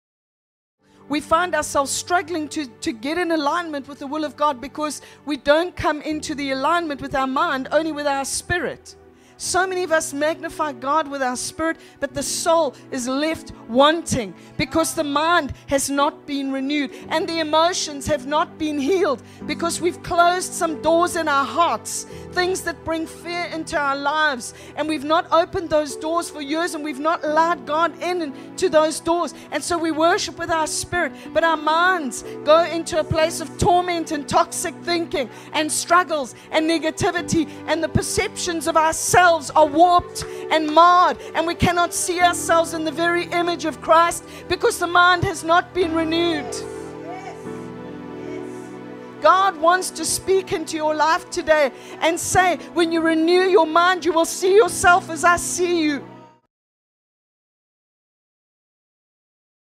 Having the Mind of Christ (Pt 2) - 1-minute Clip - Apostle Deborah Bell
Take a look at this 1-minute clip from Part 2of Apostle Deborah Bell's powerful new series, "Having the Mind of Christ" and then head for the full replay, and the rest of the series!

Deo Gloria Family Church is an affirming church.